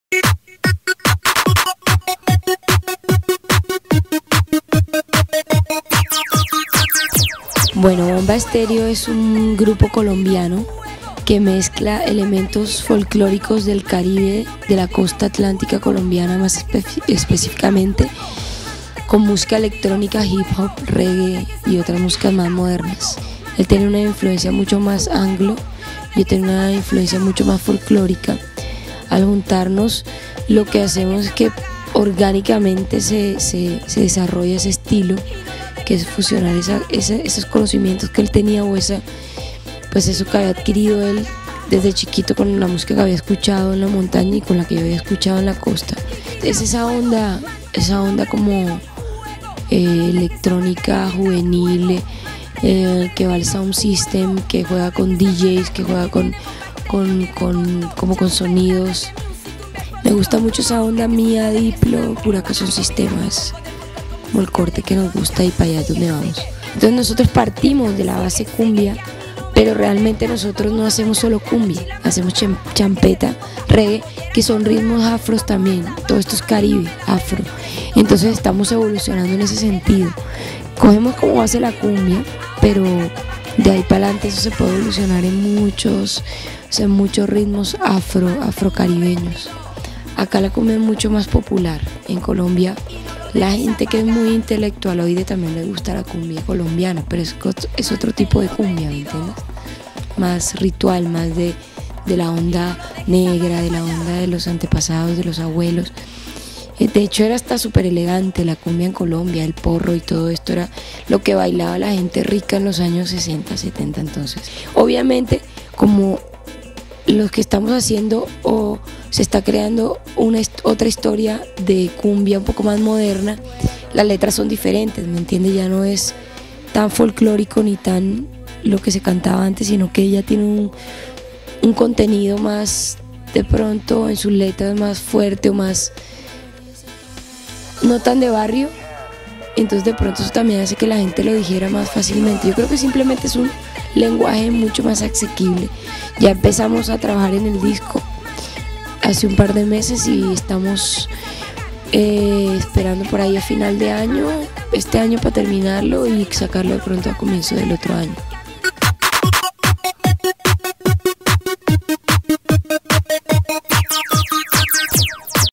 Bomba Estéreo: Todo sobre música afrocaribeña
No te pierdas esta entrevista con la vocalista de Bomba Estéreo, Liliana Saumet, donde nos habla a grandes rasgos sobre sus raíces, la champeta, la escena en Colombia, el kuduro, y todo lo que gira alrededor de este proyecto de "cumbia psicodélica" oriundo de Bogotá.